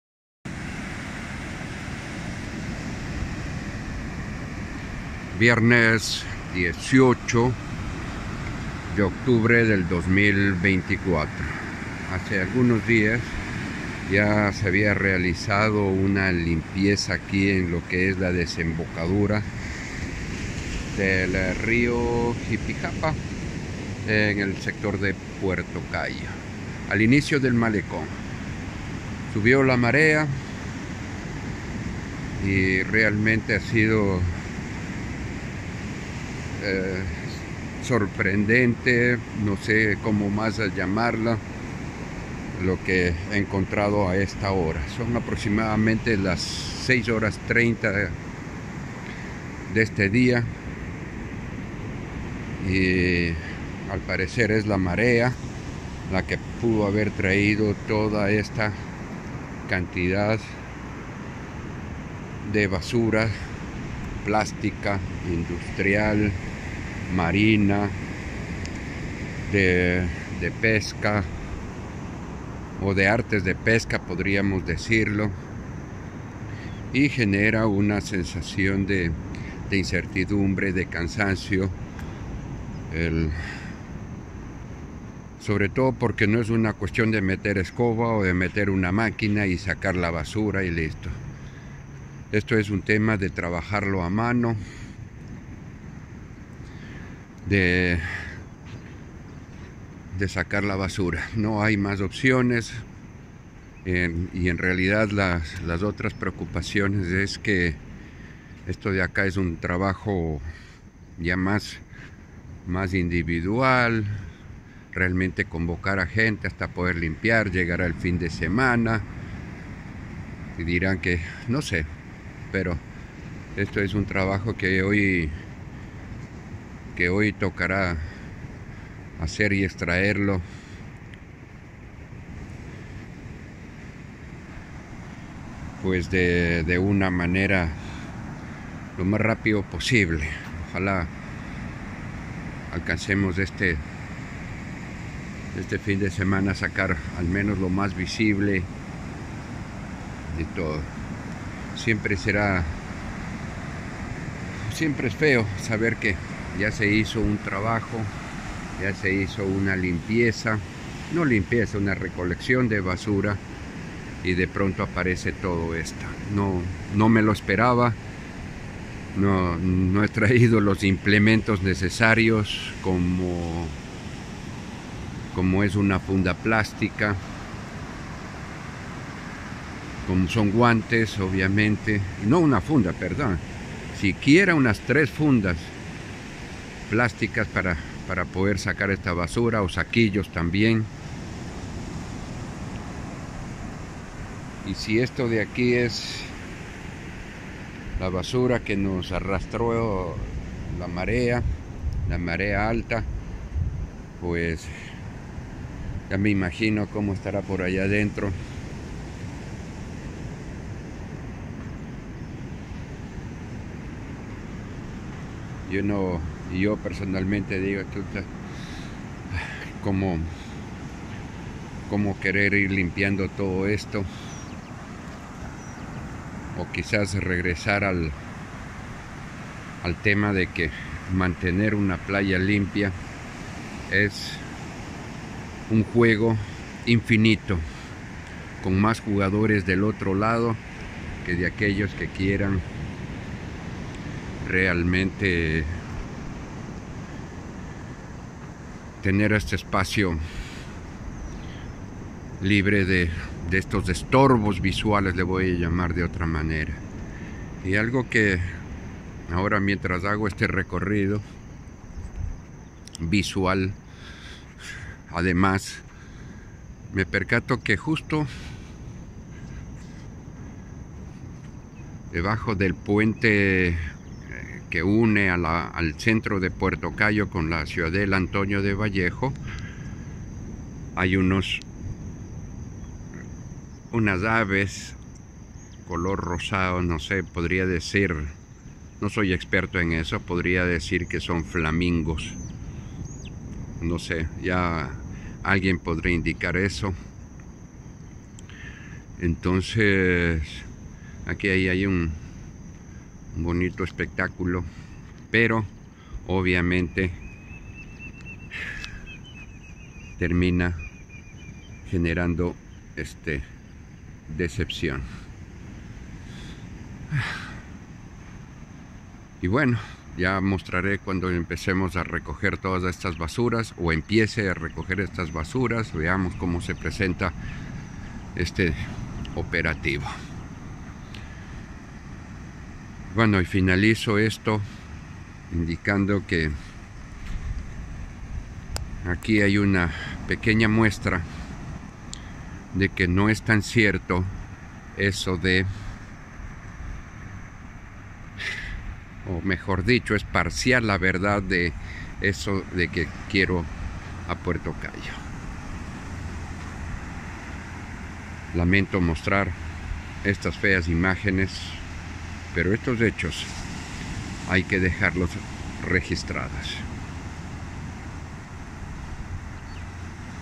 La basura que devolvió el mar
Luego del oleaje de la madrugada del 18 de octubre de 2024, fue posible observar la cantidad de desperdicios plásticos que se acumularon en la línea de marea alta en la desembocadura del río Jipijapa, sector del inicio del Malecón de Puerto Cayo.